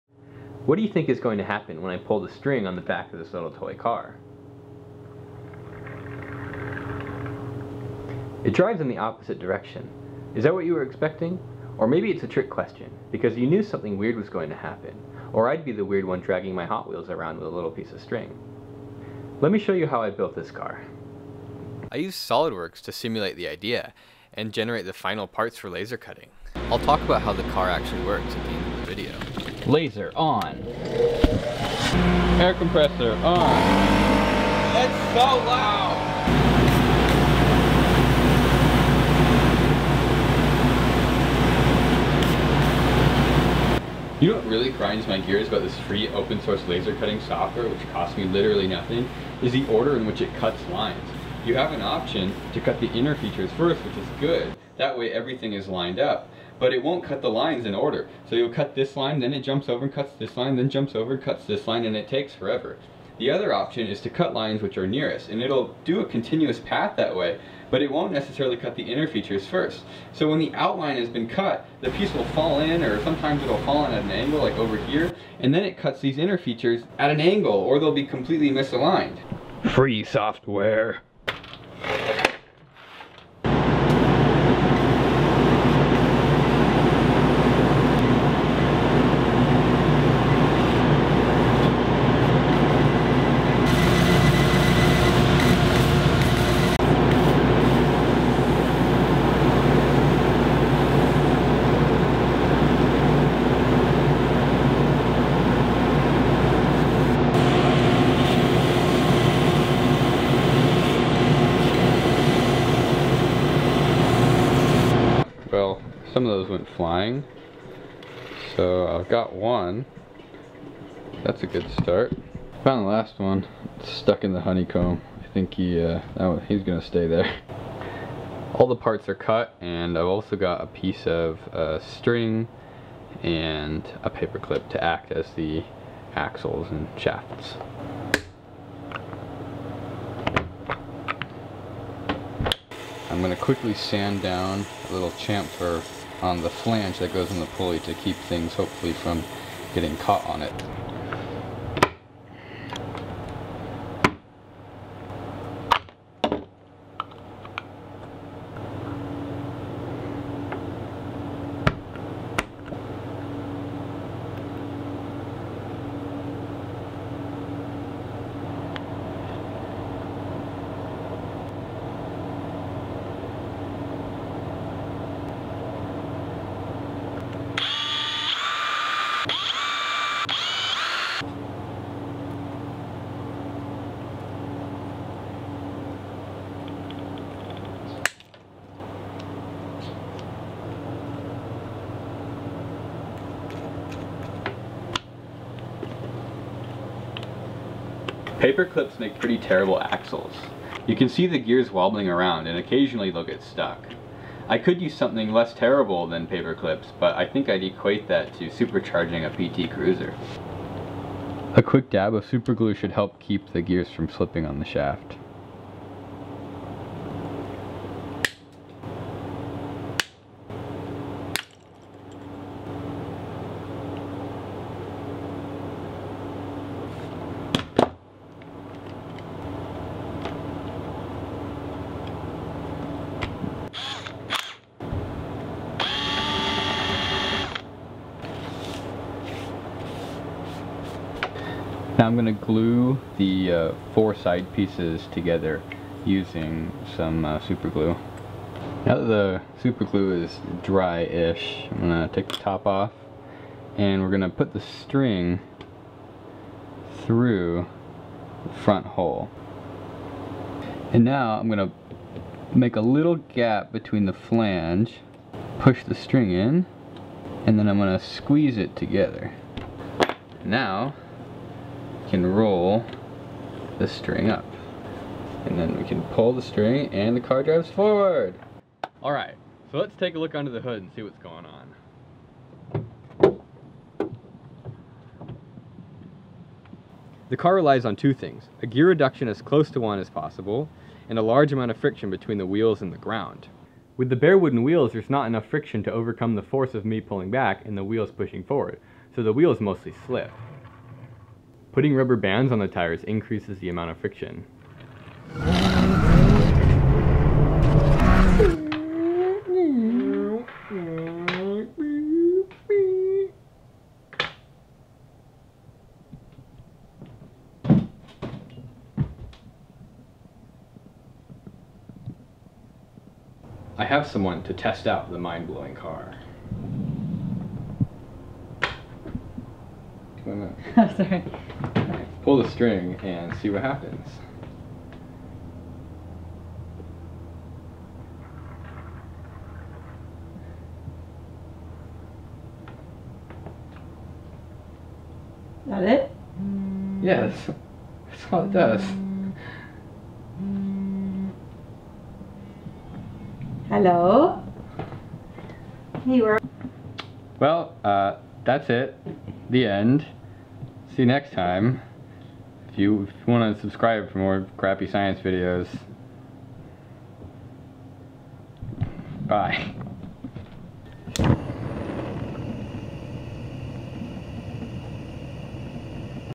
Laser Cutting an Unusual Toy
I designed and laser cut an unusual toy car.